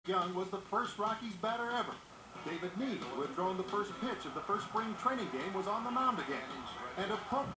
David Nied 1st Colorado Rockies Strikeout Vince Coleman
David Nied gives the Colorado Rockies their 1st strikeout when he fans Vince Coleman of the New York Mets.